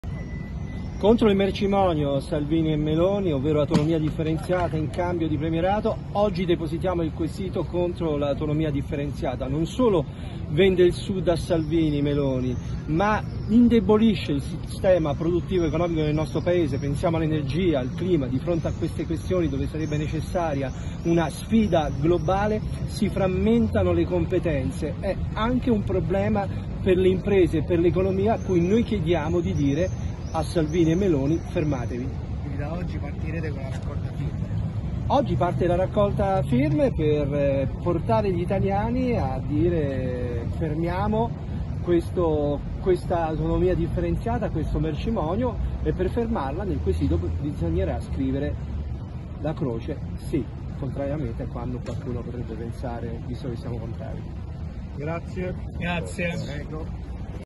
Referendum Autonomia, Bonelli: ”Meloni vende il Sud a Salvini”
Referendum Autonomia, le parole di Angelo Bonelli, deputato Avs